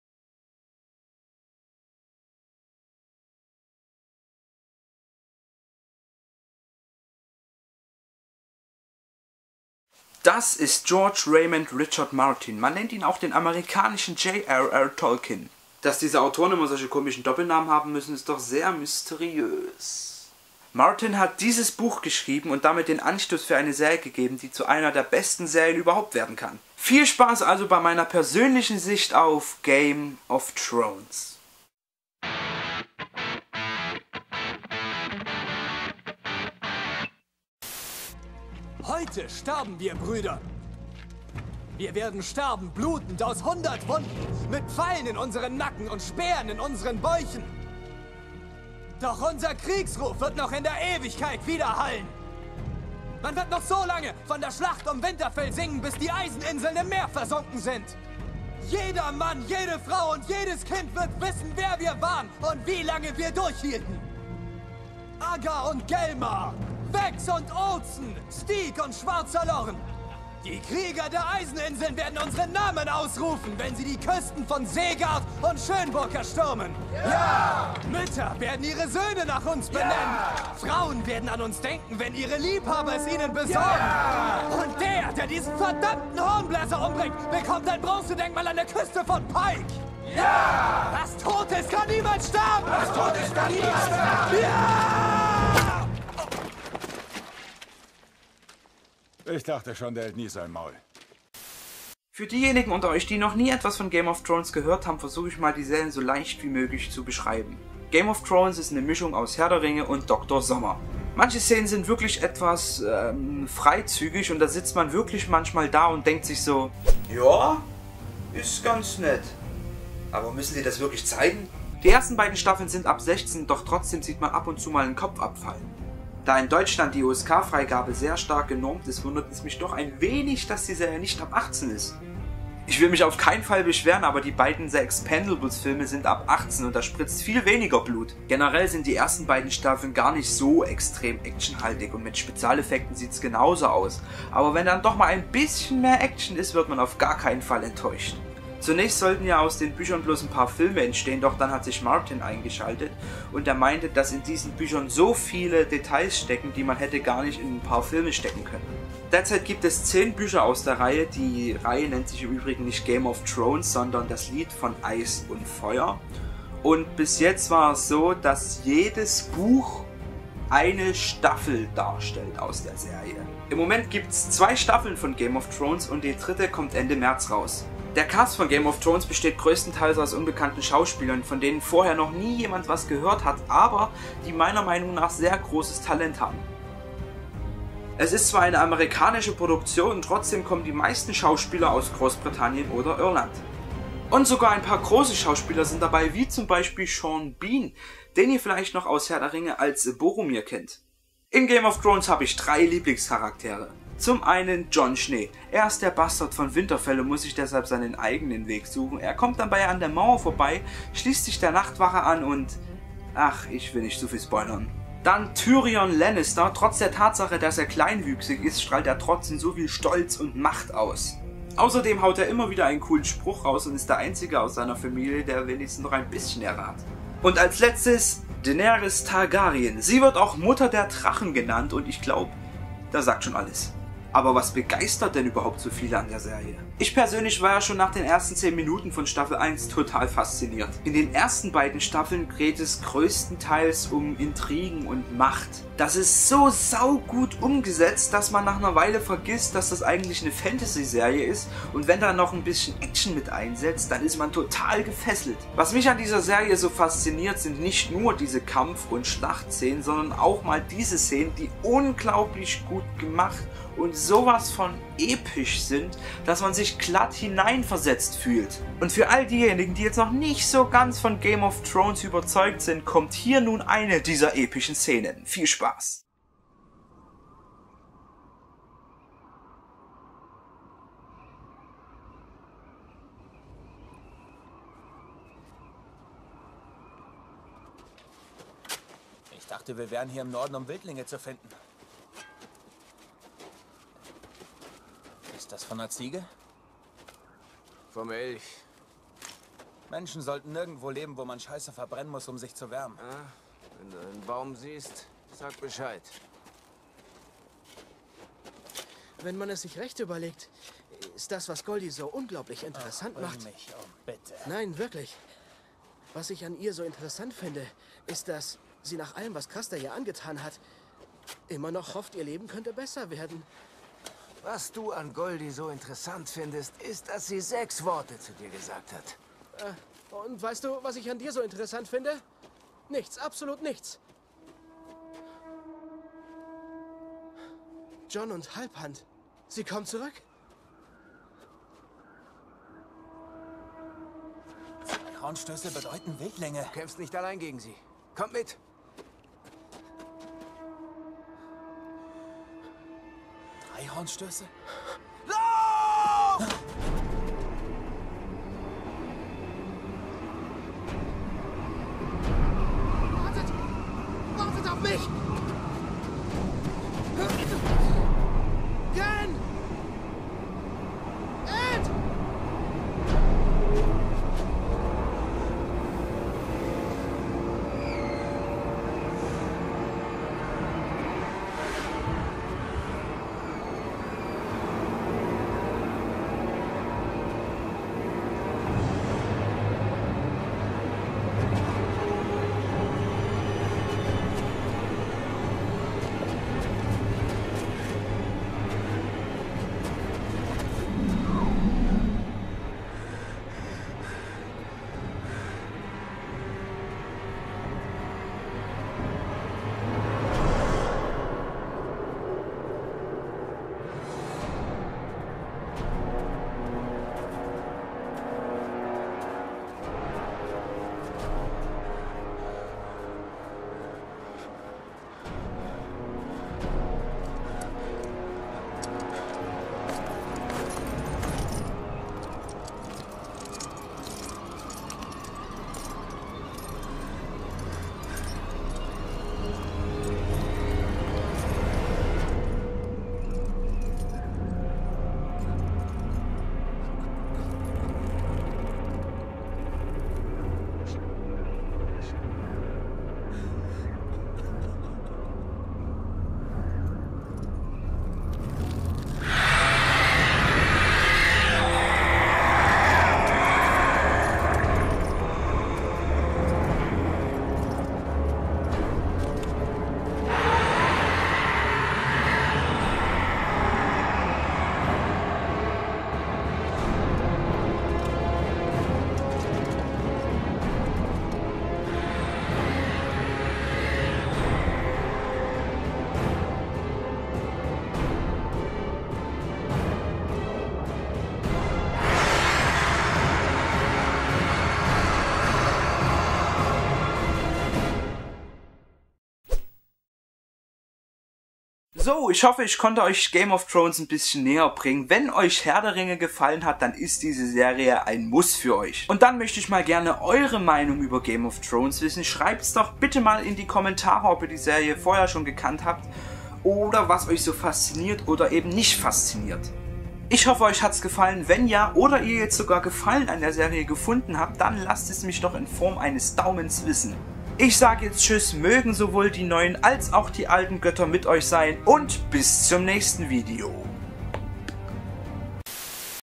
Das „Game of Thrones" Fieber
Game of Thrones ist momentan eine der erfolgreichsten Serien. Und da ich genau so begeistert von der Serie bin wie andere auch, mach ich einfach mal ein Video über Game of Thrones :D
Um vielleicht für den ein oder anderen dem der Begriff "Game of Thrones" nichts sagt, die Serie schmackhaft zu machen :)